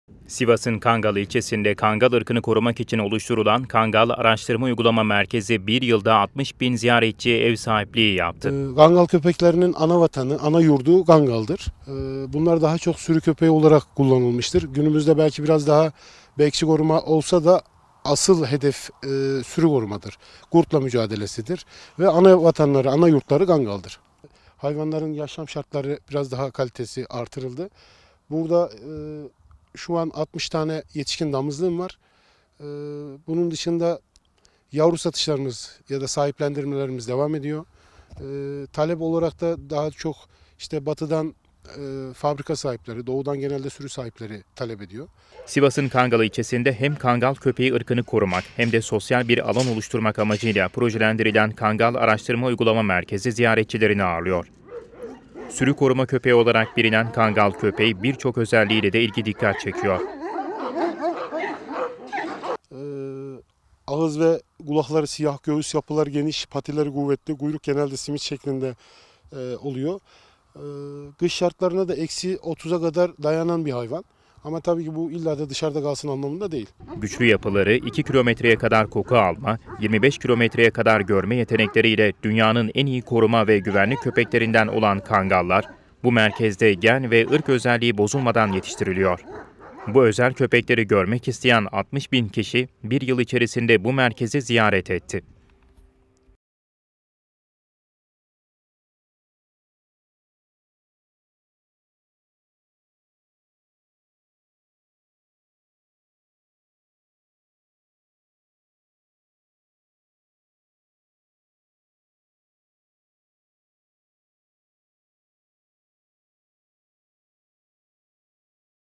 Dünyaca Ünlü Kangallar, 60 Bin Ziyaretçi Ağırladı
Sivas’ın Kangal ilçesinde, Kangal ırkını korumak için oluşturulan Kangal Araştırma Uygulama ve Dokümantasyon Merkezi bir yılda 60 bin ziyaretçiye ev sahipliği yaptı.

Sivas’ın Kangal ilçesinde hem Kangal köpeği ırkını korumak hem de sosyal bir alan oluşturmak amacıyla projelendirilen Kangal Araştırma Uygulama ve Dokümantasyon Merkezi ziyaretçilerini ağırlıyor. Sürü koruma köpeği olarak bilinen Kangal köpeği, birçok özelliğiyle de ilgi dikkat çekiyor. Güçlü yapıları, 2 kilometreye kadar koku alma, 25 kilometreye kadar görme yetenekleri ile dünyanın en iyi koruma ve güvenlik köpeklerinden olan Kangallar, bu merkezde gen ve ırk özelliği bozulmadan yetiştiriliyor. Bu özel köpekleri görmek isteyen 60 bin kişi bir yıl içerisinde bu merkezi ziyaret etti.